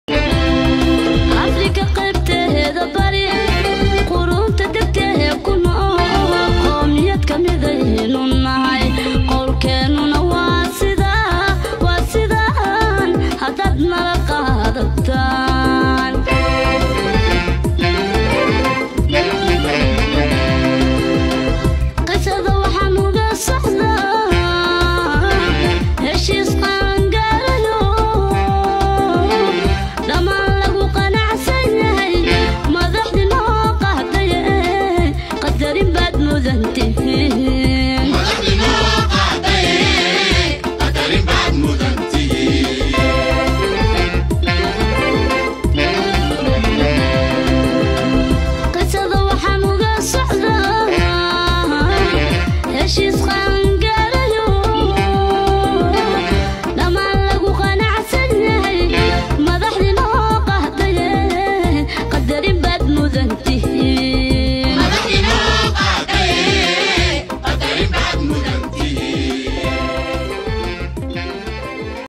Gudoomiyaha golaha shacabka Sheekh Aadan Madoobe oo soo gaaray Shirka Wadatashiga madaxdii.
Shirka Madaxweyne Xasan sheekh iyo Madaxdii hore iyo Siyaasiyiinta.